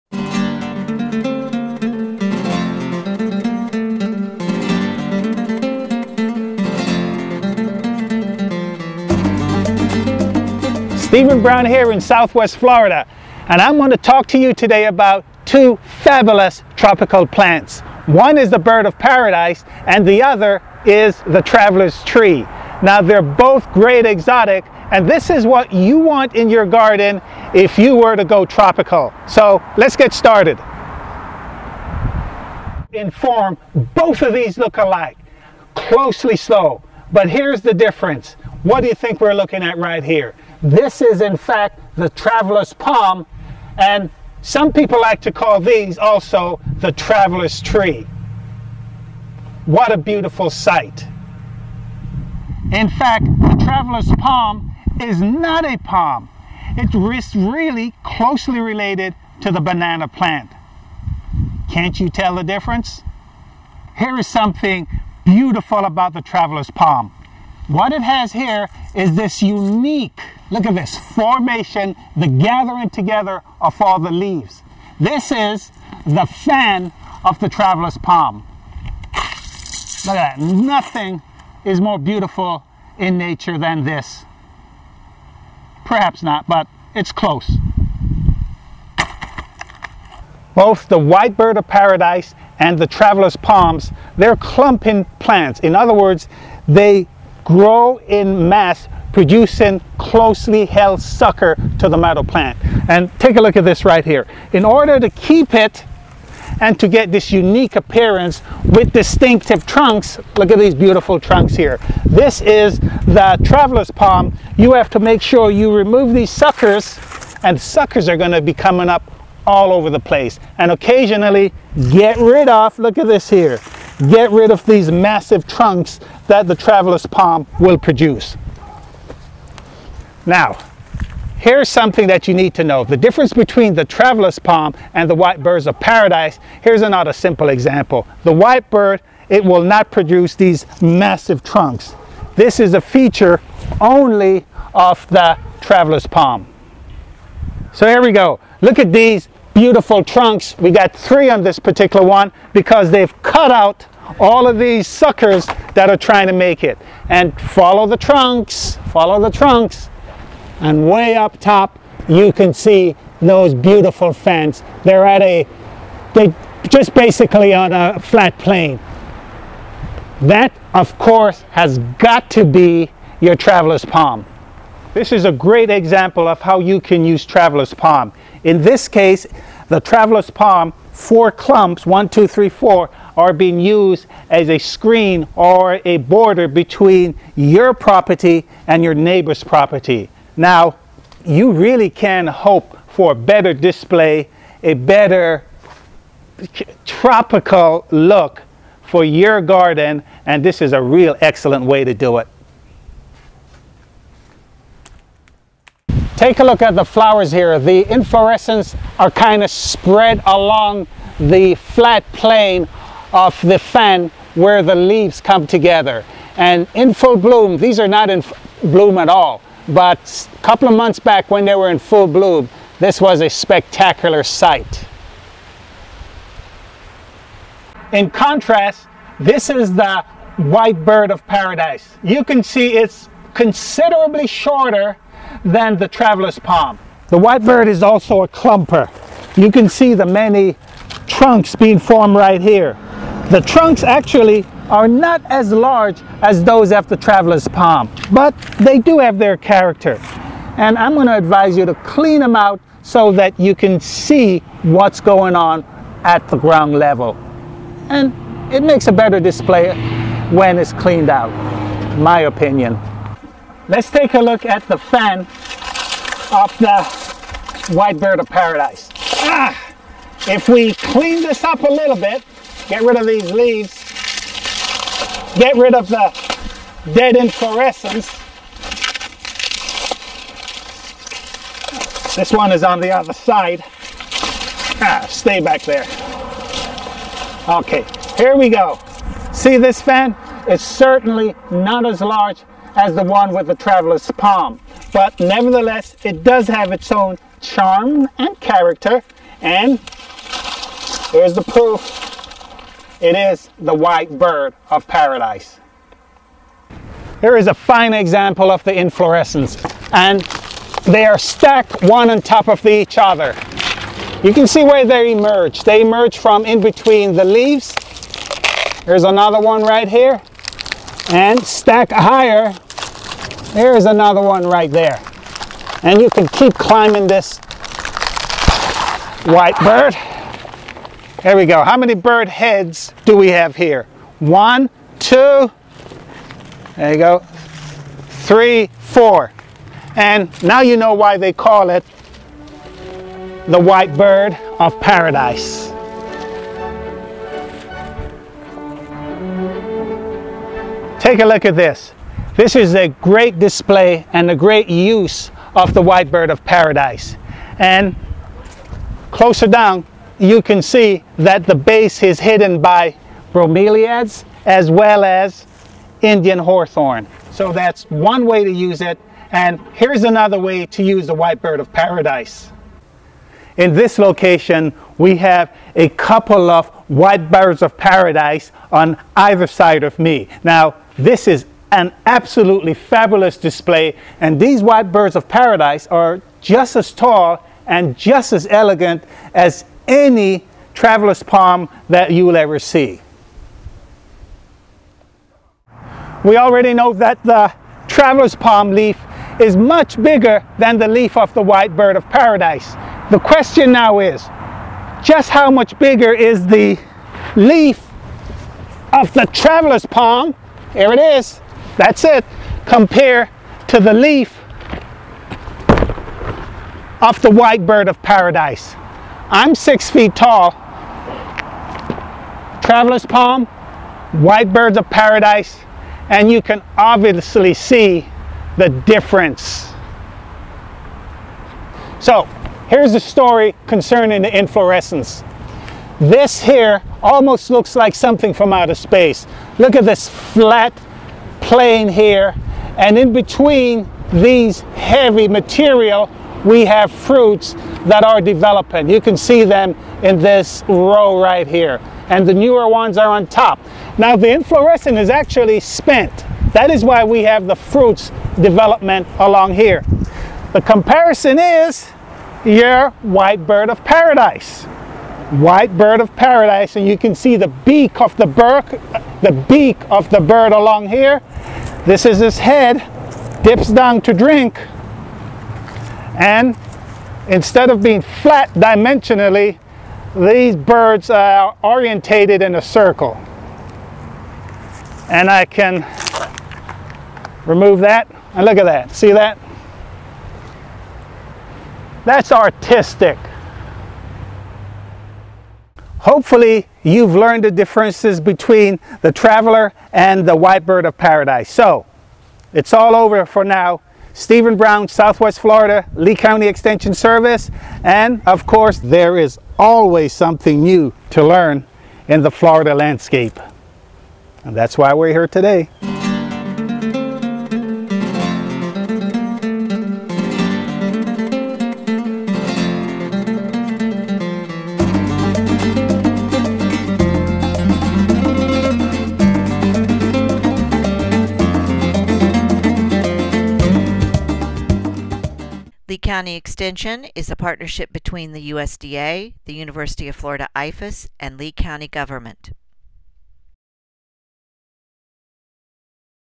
How to Identify the Traveler's Palm from The White Bird of Paradise
Learn the difference between the traveler 's palm and the white bird of paradise with Stephen Brown, Lee County Extension Agent.